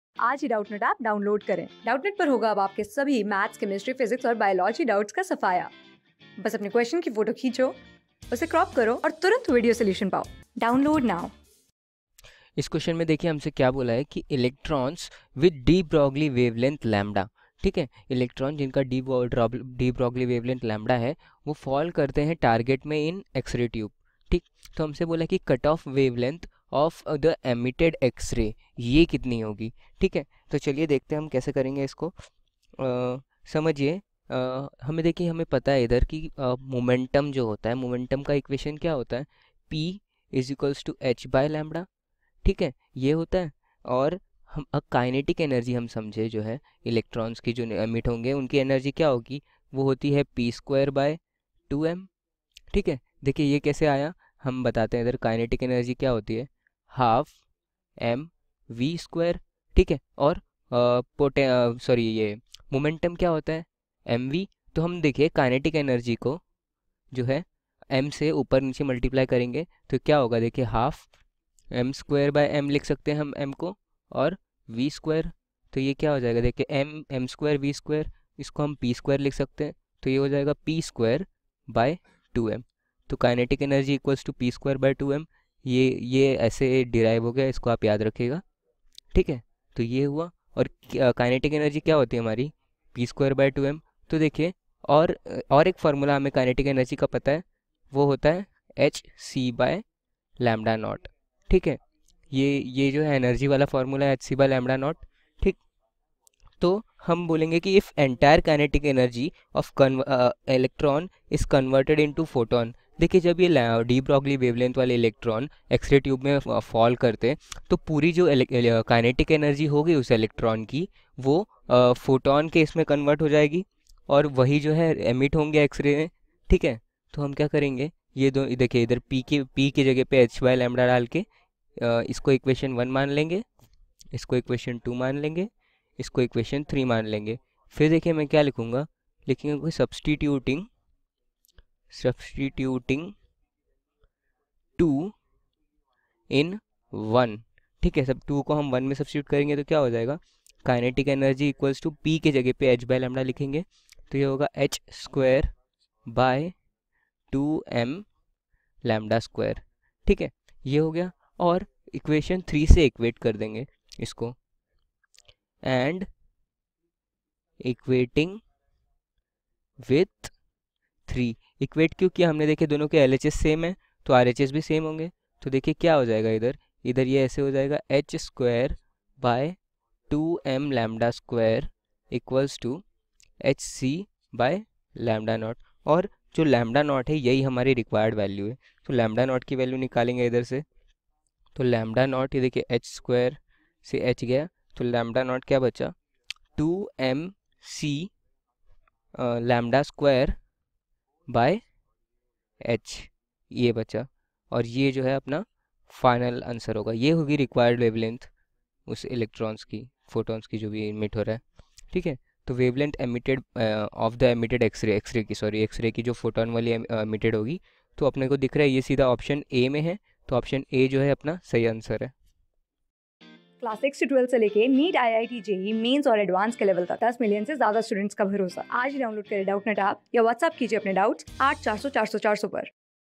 Electrons with de-Broglie wavelength `lambda` fall on the target in an X-ray tube. The cut-off
Electrons with de-Broglie wavelength `lambda` fall on the target in an X-ray tube. The cut-off wavelength of the emitted X-ray is